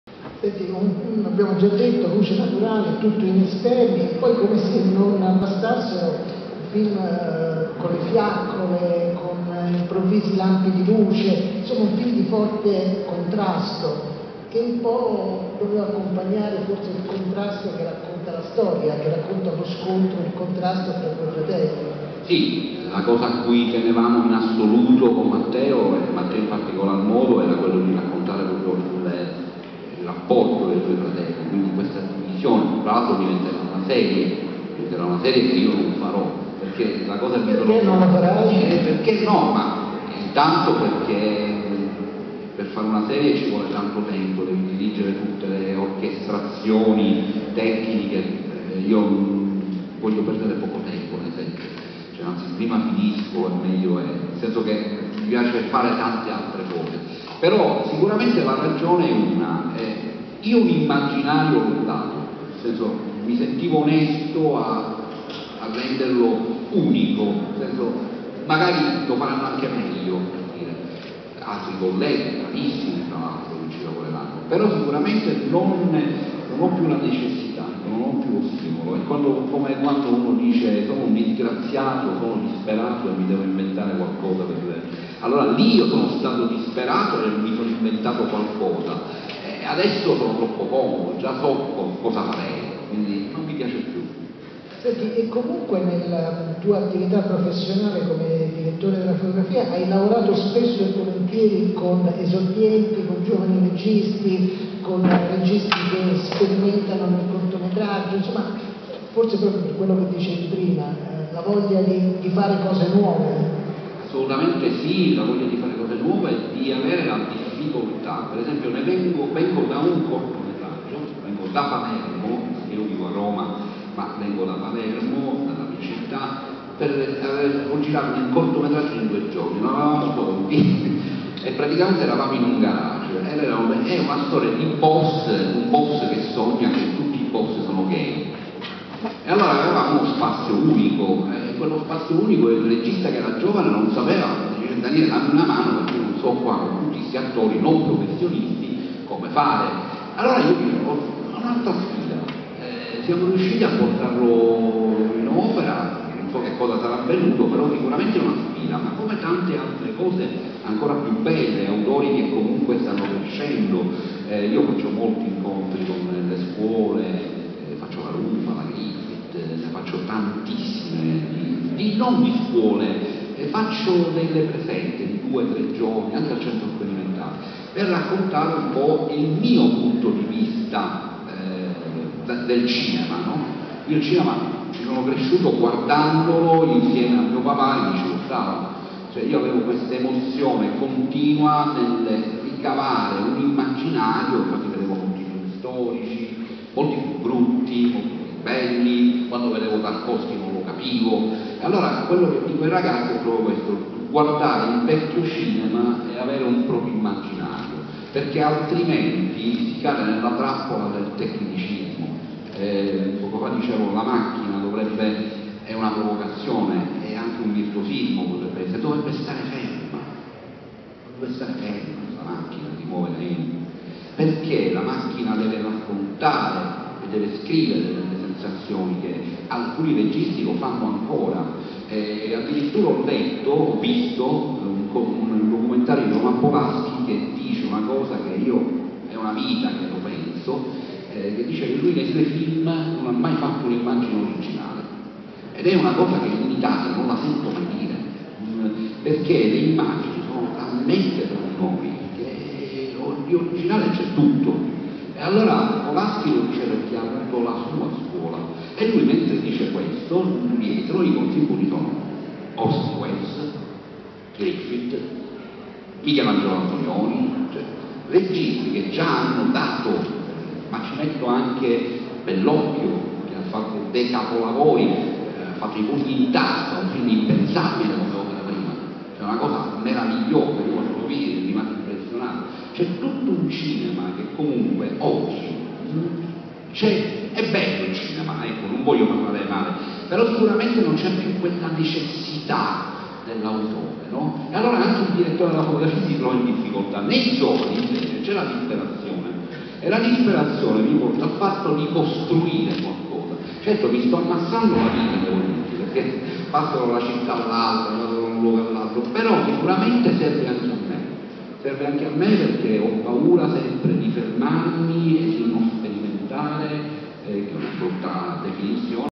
Daniele Ciprì al BIF&ST 2019: la sfida del film Il primo re
- L'obiettivo di Daniele Ciprì è stato raccontare il rapporto ed il contrasto tra i due fratelli, fulcro principale del film, anche con la luce. In questo video l'opinione del Direttore della Fotografia, al BIF&ST 2019 per ricevere il premio alla miglior fotografia, che conferma di non aver voluto proseguire a lavorare nell'omonima serie perché ama fare sempre cose nuove. Infatti lavora spesso con giovani esordienti che per lui sono sfidanti e gli piace molto insegnare le sue esperienze anche nelle scuole